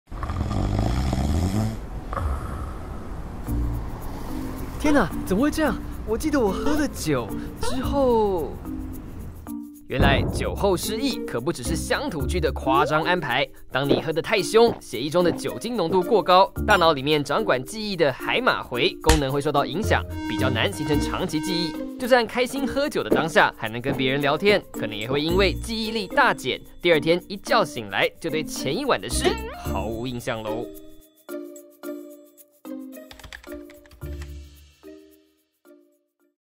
酒後失憶可不是演的！是因為大腦也醉了
「怎麼可能！你說你喝了酒，之後的事就不記得了？」但其實「酒後失憶」未必是虛構出來的誇張說法，而是真有其事。不過究竟「喝酒」與「喪失記憶」之間有什麼關聯？來瞧瞧動畫就知道。

請支持我們讓更多人看到，來為你喜歡的影片製作外語字幕